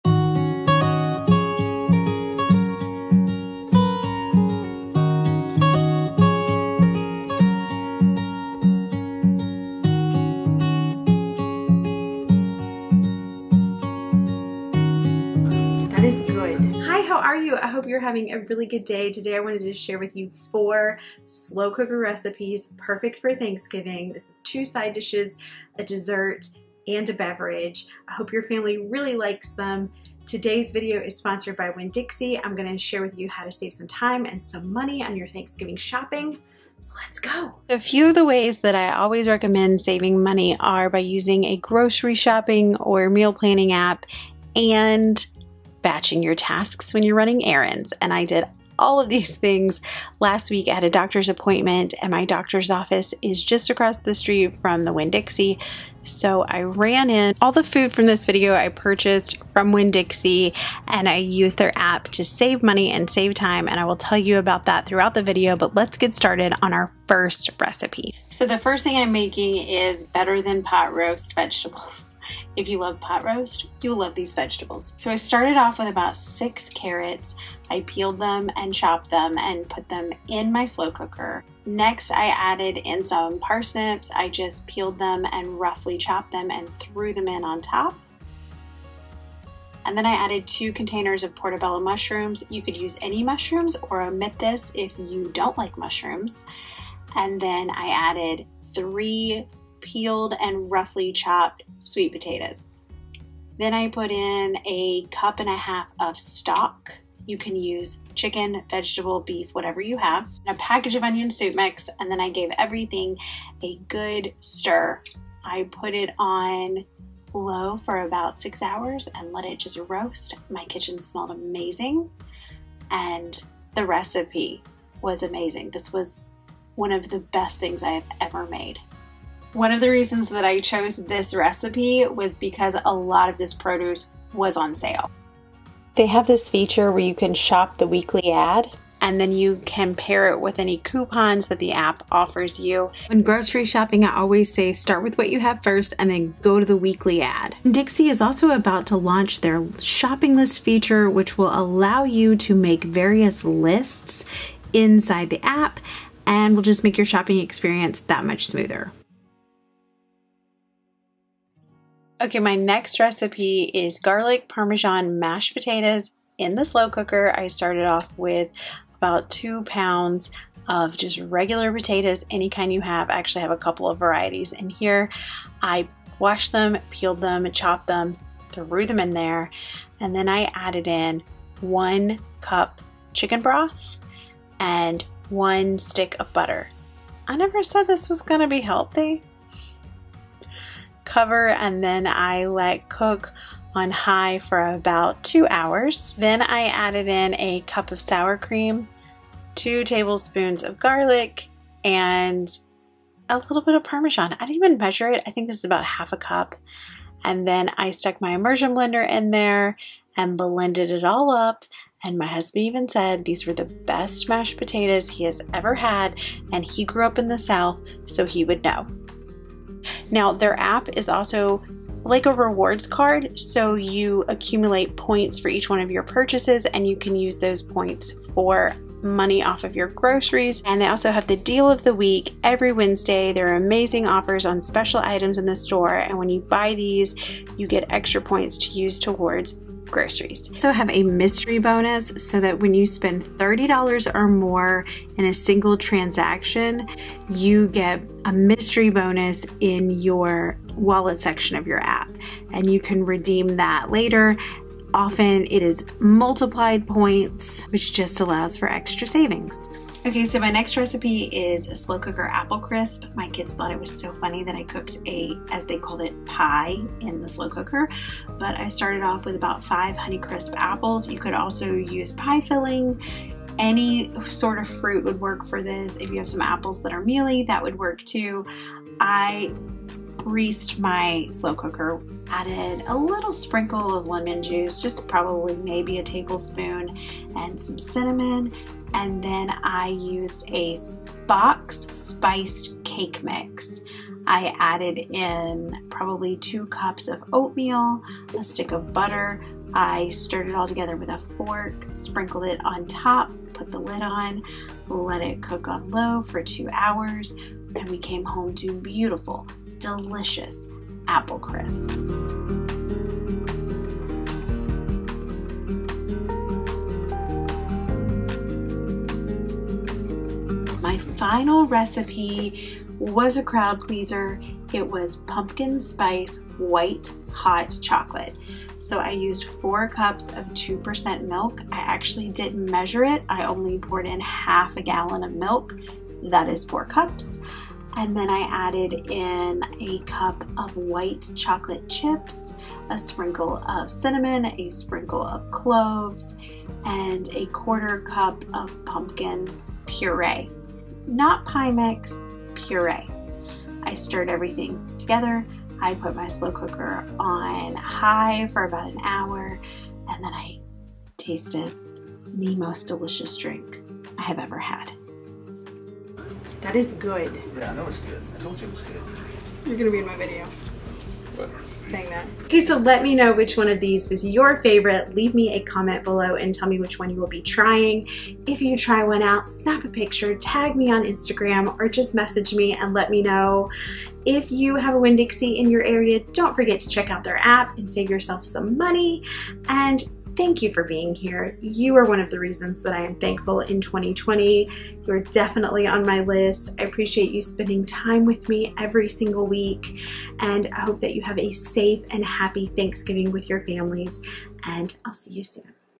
Four easy slow cooker recipes, side dishes, desserts and beverage
Today I’m sharing four easy slow cooker recipes for fall.

Snail Mail:
Lydia Senn
Creola, AL 36525